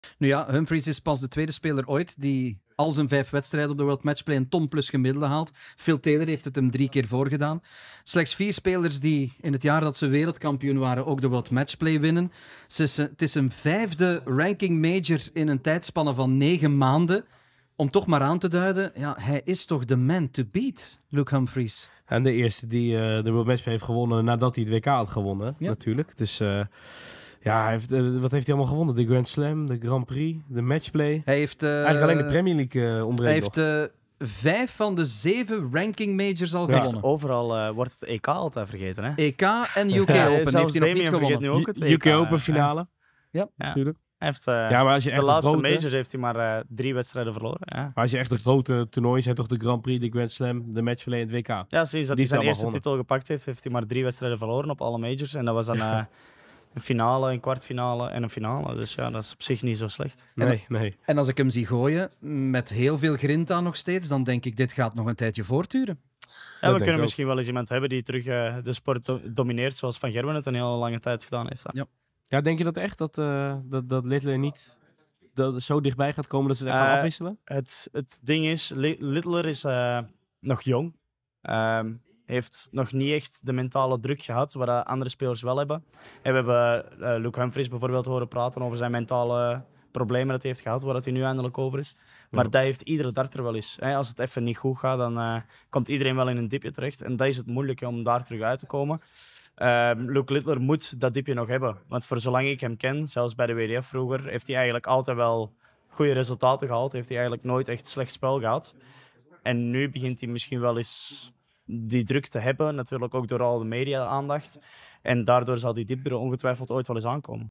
‘Die dip van Luke Littler gaat er ongetwijfeld aankomen’
We eindigen ons tweede seizoen met een knaller. Zo vertoeven Damian en Matthias in Dartshop Madhouse te Dendermonde, passeren verschillende gasten de revue en dit allemaal onder het goedkeurend oog van heel wat aanwezige kijklustigen.

Als allereerst wordt er bijgepraat met de grote baas van Dartshop Madhouse, Andy Vanhove. Daarna komt oud-profwielrenner en groot dartsfan Kevin van Impe aan het woord. Om vervolgens tourkaarthouder Robbie Knops te laten aanschuiven.

De tweede helft van de podcast schuift Brian Raman aan. ‘The Riddler’ was eerder al eens te gast bij Damian en Matthias. Nu blikken zij vooral terug op de World Matchplay.

Damian en Matthias doen ter afsluiting nog wat aankondigen en bedanken ook hun gloednieuwe vaste sponsor, de Goudenregenbar uit Den Haag.